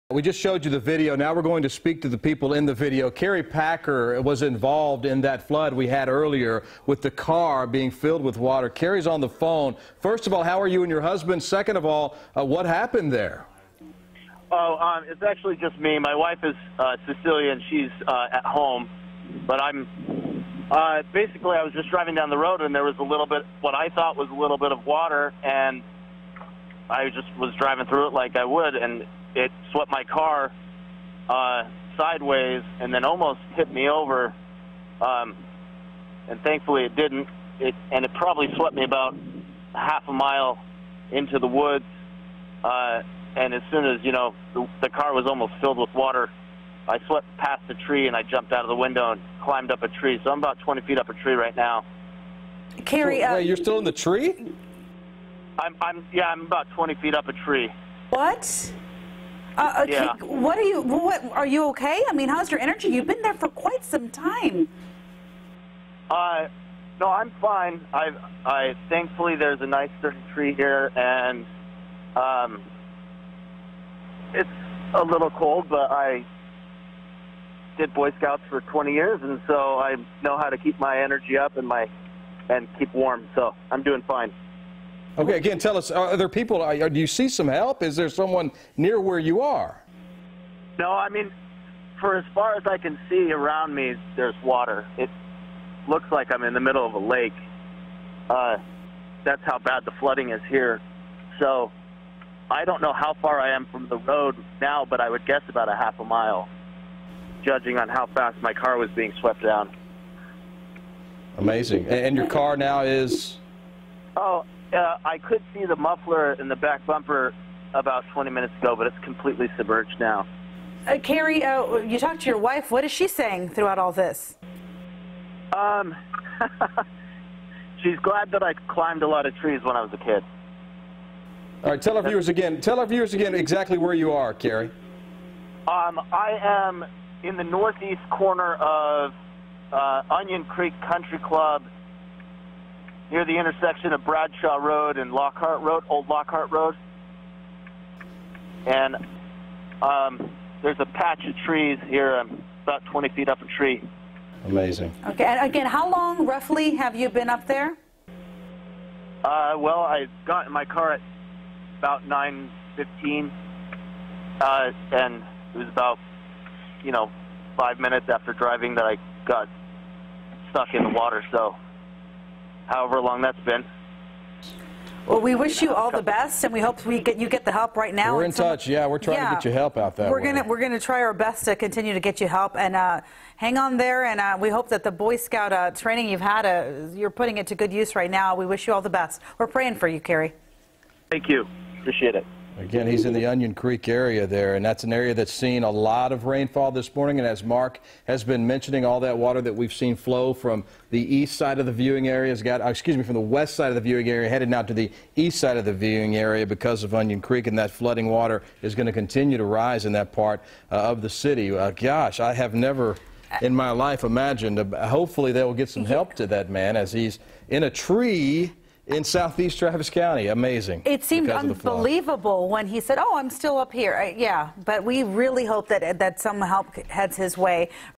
ON THIS DAY: Kerry Packer in a tree | Oct. 30, 2015
Oct. 30, 2015: Heavy rain brought flooding to the Austin area, and Kerry Packer was forced to climb a tree after flooding washed his car away near Onion Creek.
Packer was rescued after more than four hours in the tree, but his interview (and Bryan and Yvonne’s reaction) are worth watching again.